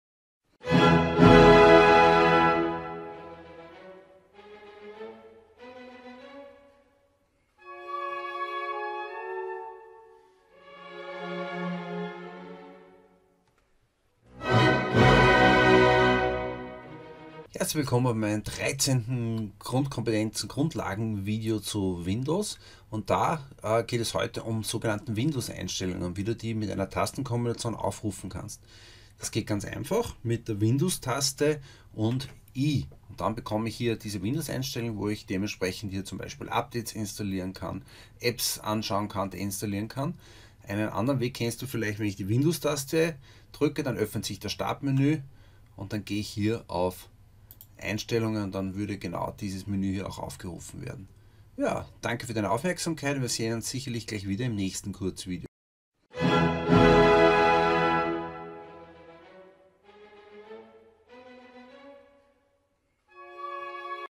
13 Mit einer Tastenkombination Windows – Einstellungen öffnen
Ich zeige hier, wie man die Windows Einstellungen mit einer Tastenkombination öffnen kann. Dieses Video ist Teil meiner Playlist Windows 10 Grundkompetenzen / Grundlagen, die sich an Einsteiger*innen in das Betriebssystem richtet.

Hier ist der Link zur Playlist mit 46 Videos zum Thema Windows 10: